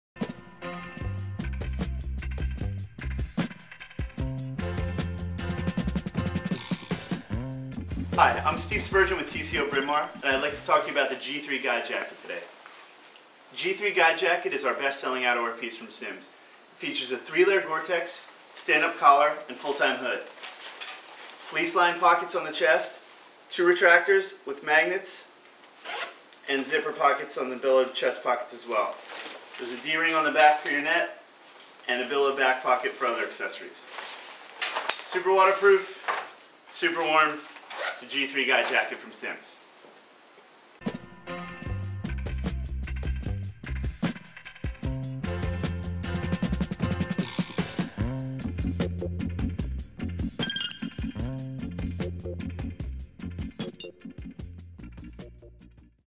TCO Fly Shop - Simms G3 Jacket
Steve Spurgeon - TCO Bryn Mawr, demostrates the features of the Simms G3 Jacket.